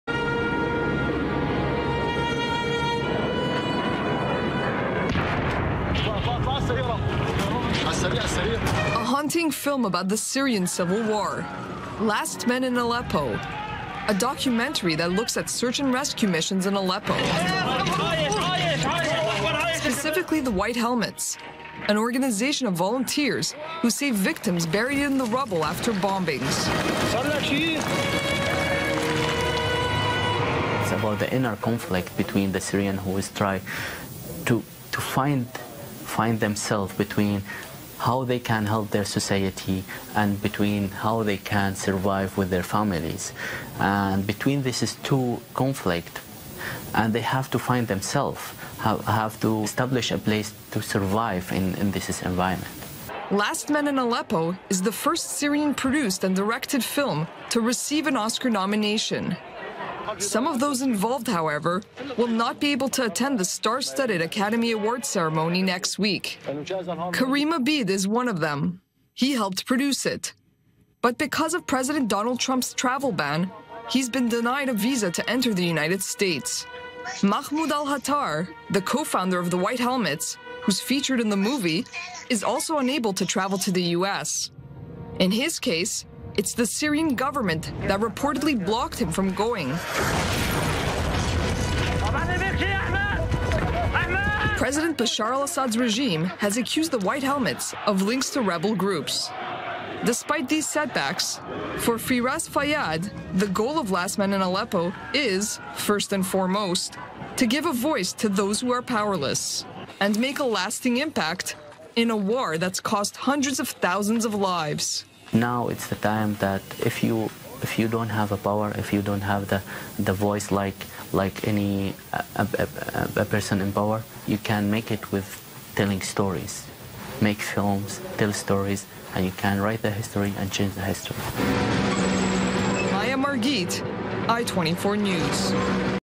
Last Men in Aleppo: Syrian documentary hopes to write history
'Last Men in Aleppo’ is an award-winning documentary film that looks at a volunteer rescue group in Syria known as the White Helmets. Now it’s also up for an Oscar, but some of the filmmakers won’t be able to make it to the red carpet in Hollywood next week. Culture Correspondent Maya Margit with more.

Edited by Victoria Uchitel